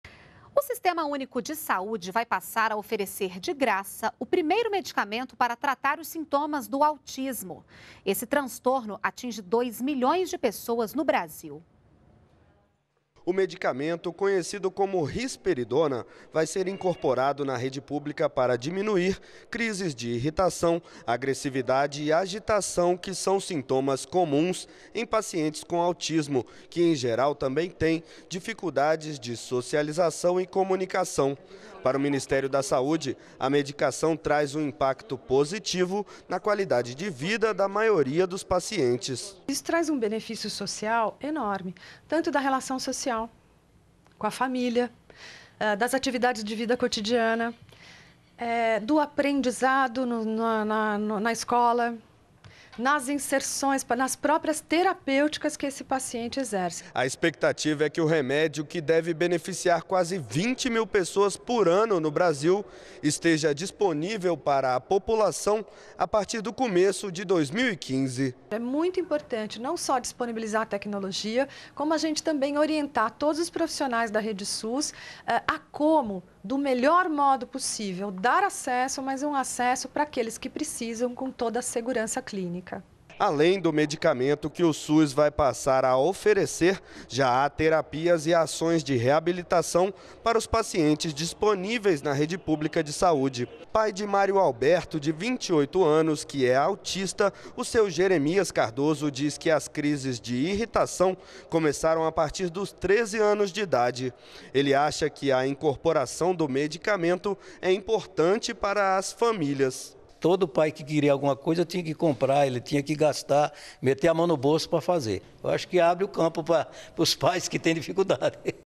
Medicamento que trata sintomas do autismo vai ser oferecido gratuitamente pelo SUS
NBR NOTÍCIAS - 19.09.14: O medicamento Risperidona, usado para tratar os sintomas do autismo, vai ser oferecido, de graça, pelo Sistema Único de Saúde (SUS), a partir de 2015. Esse transtorno, que atinge dois milhões de pessoas no Brasil, se caracteriza por quadro de estresse, irritação, agressividade, agitação, dificuldade de socialização e comunicação.